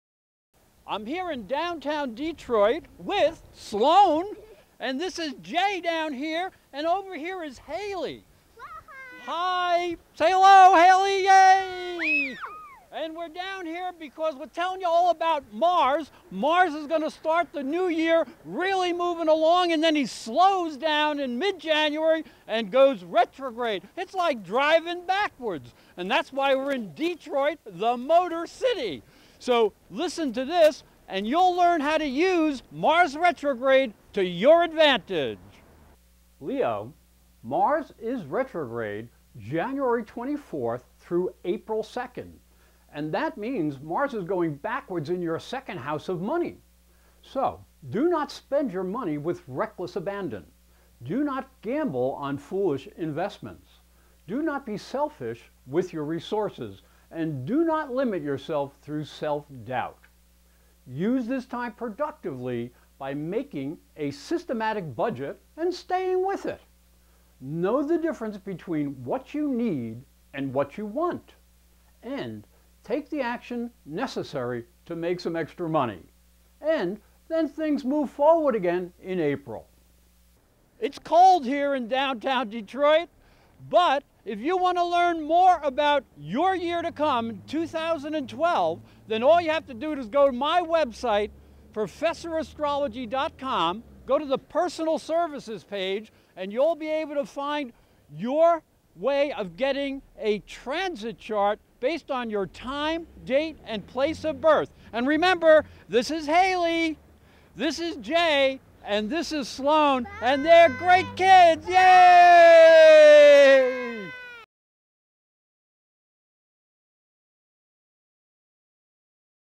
Leo Astrology January 2012
presents Leo Astrology 2012 Horoscope - Leo, Mars transits Virgo and goes retrograde in your second house from January 24, 2012 till April 14, 2012.   This is an important time for you to reorganize your spending and investment practices.  Be prepared to make a change in the way you value your talents and abilities.  They may be worth more than you think.  Here is the astrological horoscope and prediction for Leo for January 2012.  You can register for free astrology predictions at ProfessorAstrology.com This movie was filmed in Detroit, Michigan.